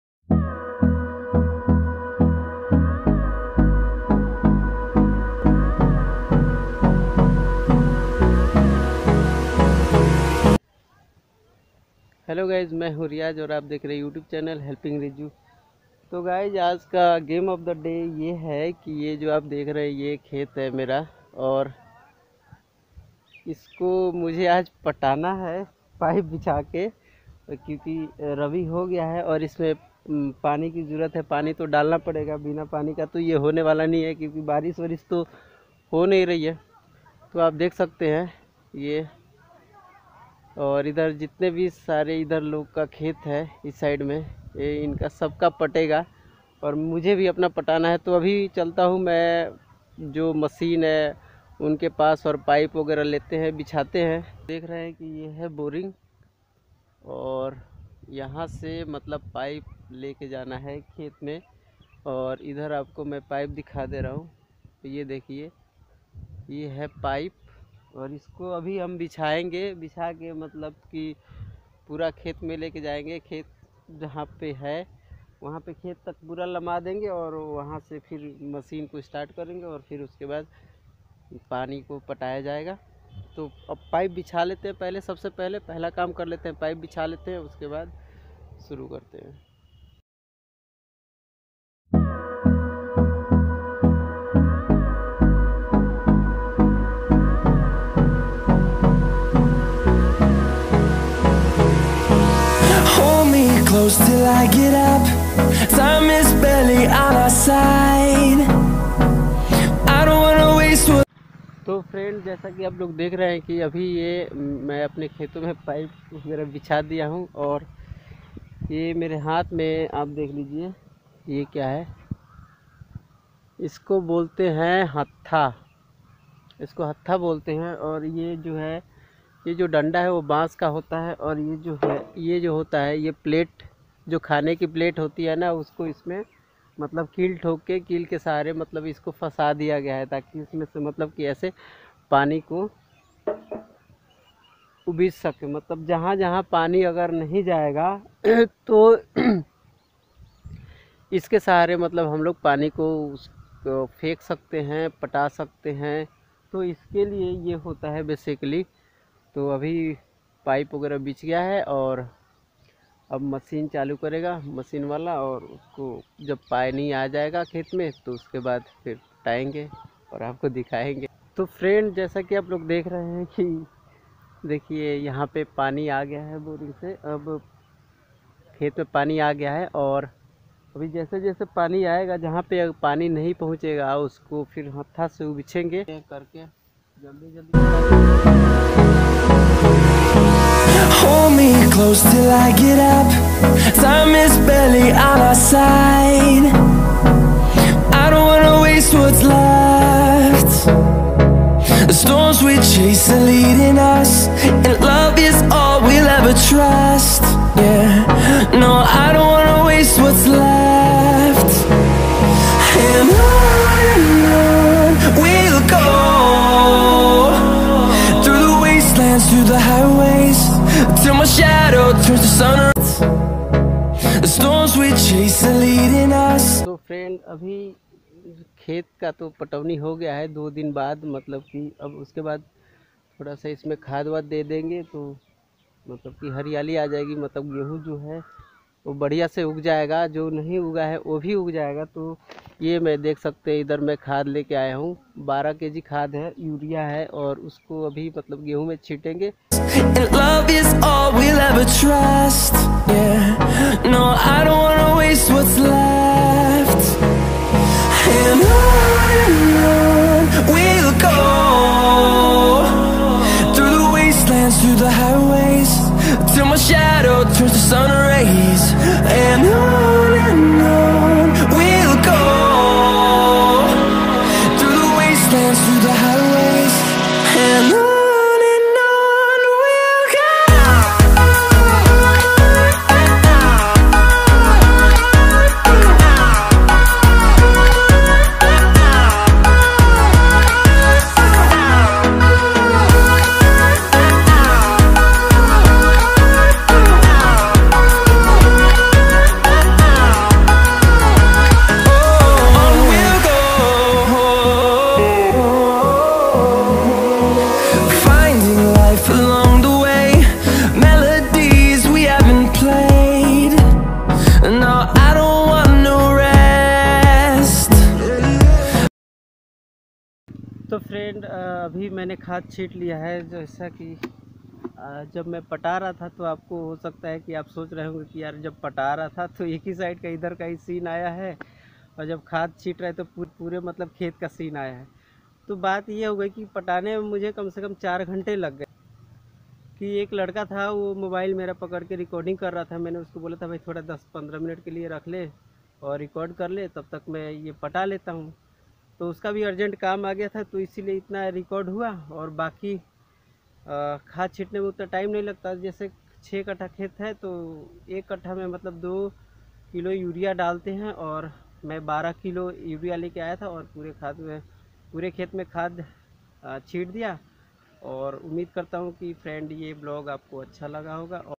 गेँहू की पटौनी || genhu ki patauni vlog || by Riyaz malik
Hlo friend mera name  Riyaz Malik  hai or apka is channel pe bahut bahut swagat hai ,aaj ke is video me Maine apko dikhaya  hai or bataya hai genhu ki patauni ke baare me  ,I hope ki ye video apko pashand aayegi.